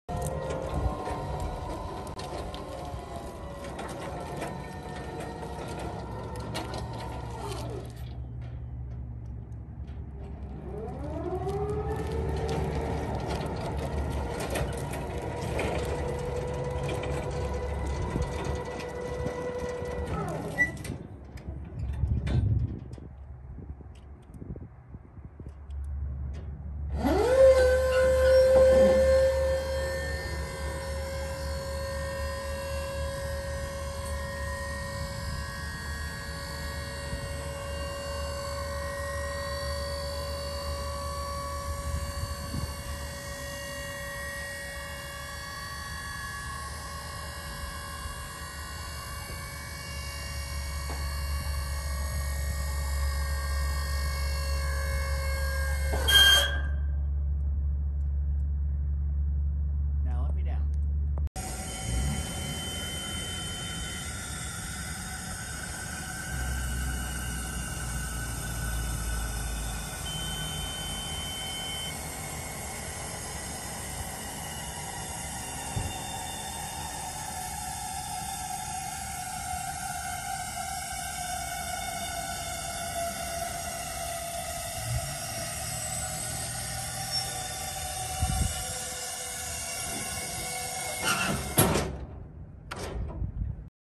2012 JLG 2030ES scissor lift for sale at auction | bidding closes December 27, 2021
2012 JLG 2030ES  scissor lift
 Hours: 215 on meter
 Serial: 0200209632
Electric Motor
 Volts: 24
Operational
 Maximum lift capacity: 800 lbs
 Maximum lift height: 20'
 Platform: 90" L x 30" W
 AC power outlet
 Controls: Platform, Ground
 Platform extensions: Manual
 Safety alarm
Tires
 Tire size: 406x125
 Solid